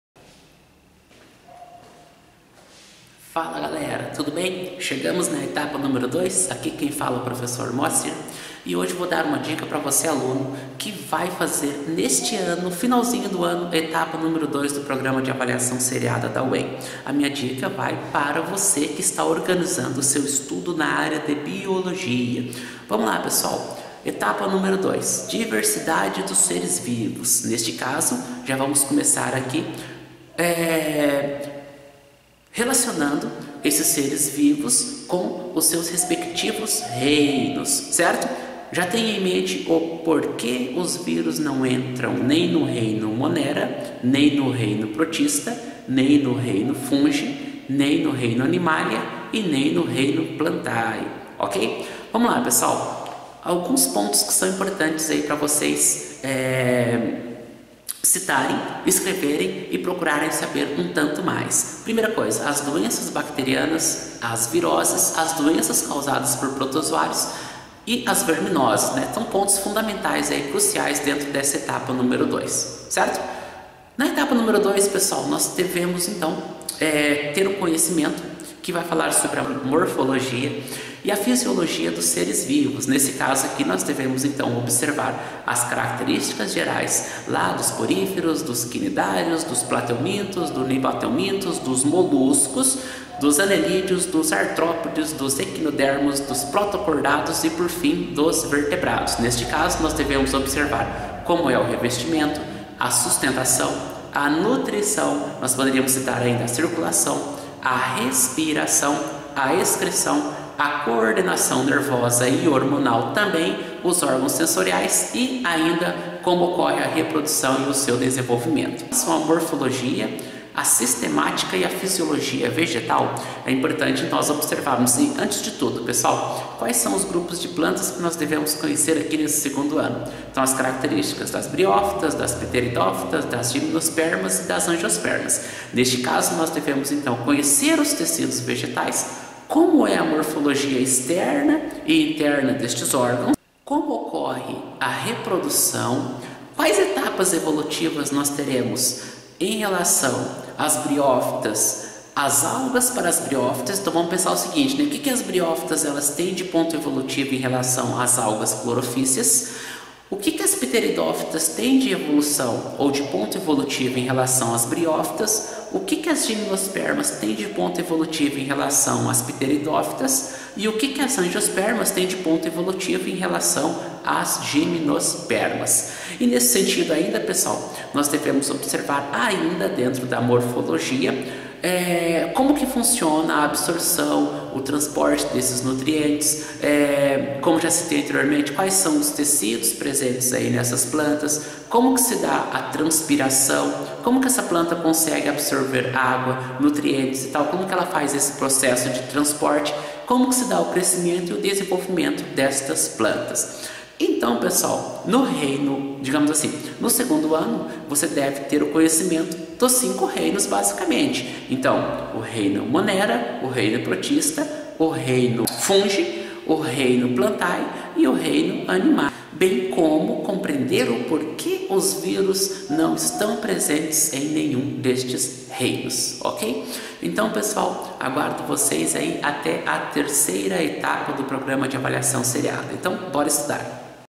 Roteiro de Estudos de BIOLOGIA PAS UEM Etapa 2
Funções Orgânicas Oxigenadas: Ácidos Carboxílicos
Relação entre a Química e a Biologia
Ácidos carboxílicos e a Fadiga Muscular
Professor: Moacir T. de Santana
Colégio Est. Humberto de Campos/ Denise C. Albuquerque/ Atalaia/Flórida
Graduação em Ciências Biológicas/ Graduação em Química
Especialista em Biologia/ Mestrando em Química